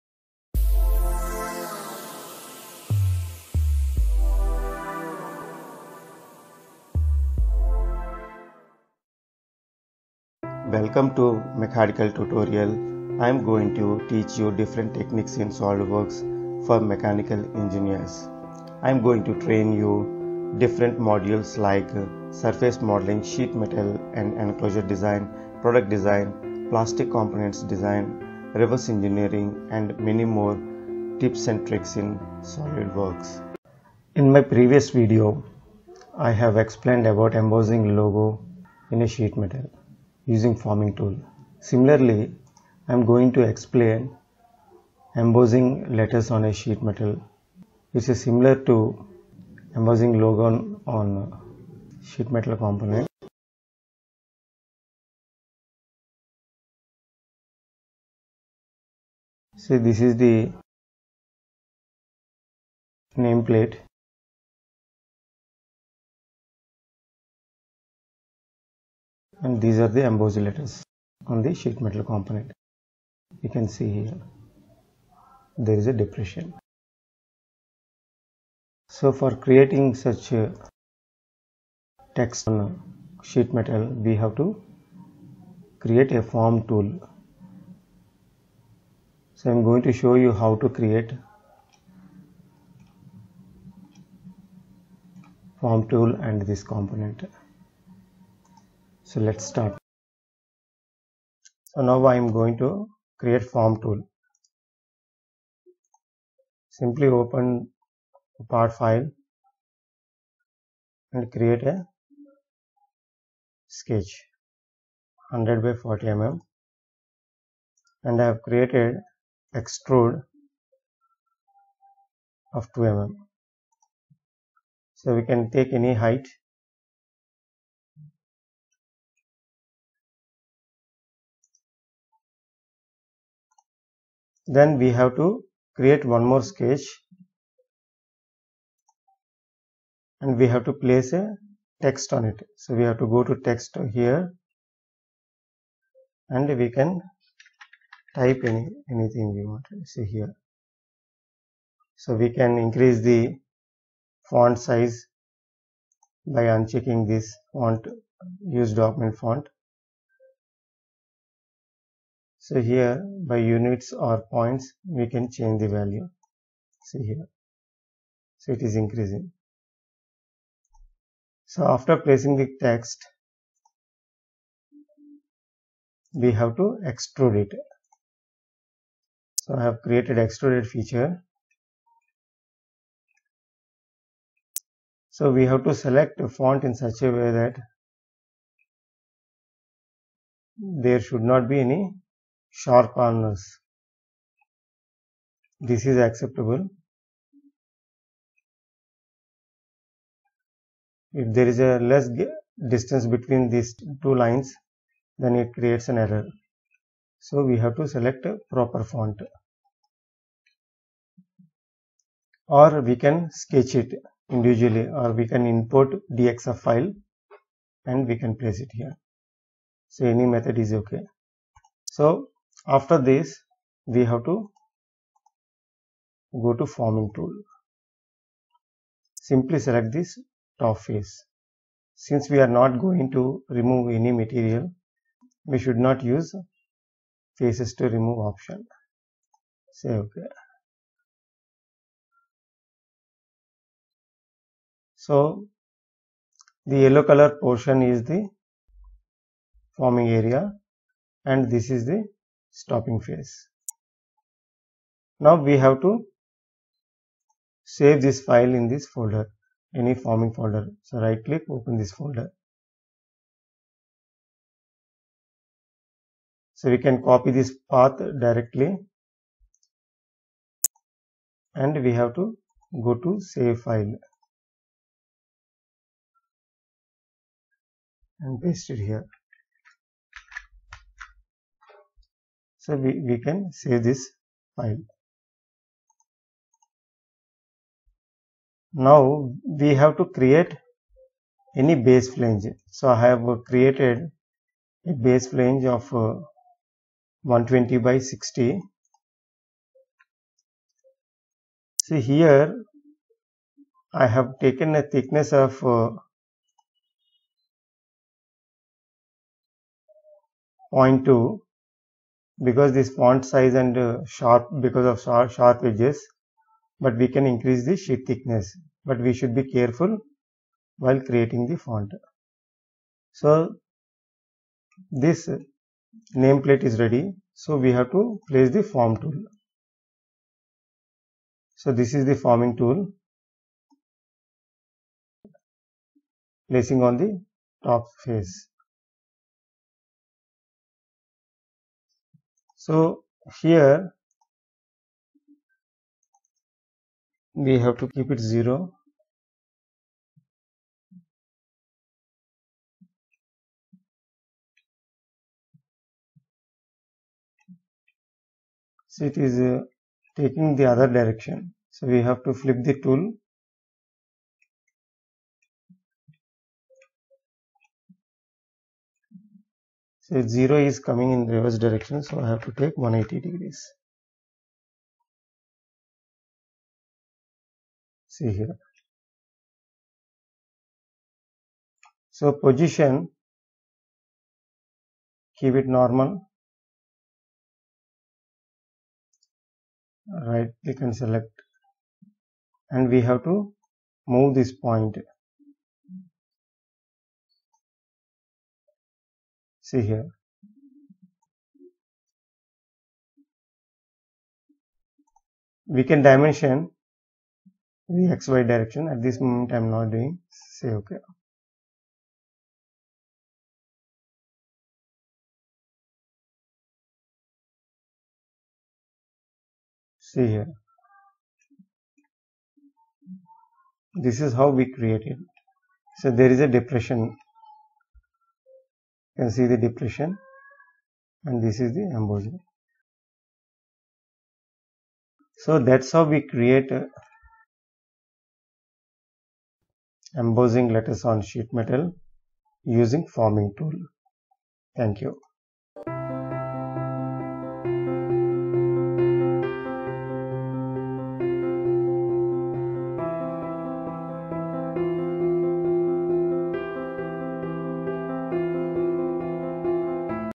Embossing Letter/Text on a Metal Sheet In SolidWorks Sheet Metal
In this video, I am going to explain about "Embossing Letter/Text on a Metal Sheet" by using SolidWorks. I am  going to explain how to create Sheet Metal Features like Base Flange, Forming Tool for Letter/Text.